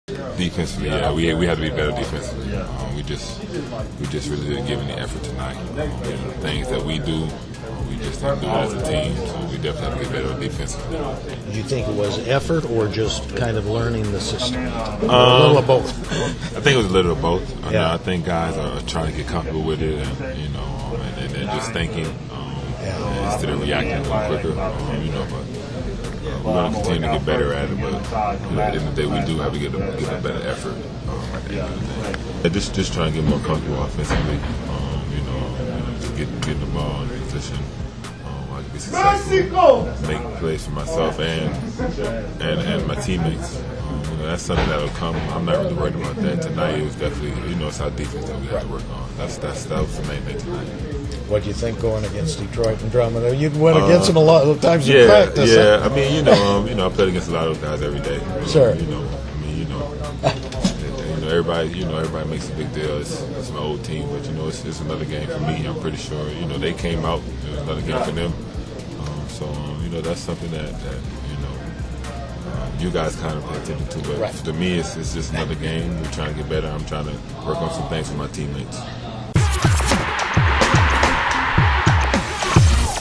Greg Monroe: We have to play better defensively
Greg Monroe spoke with the media following the Bucks' 117-88 preseason loss to the Detroit Pistons.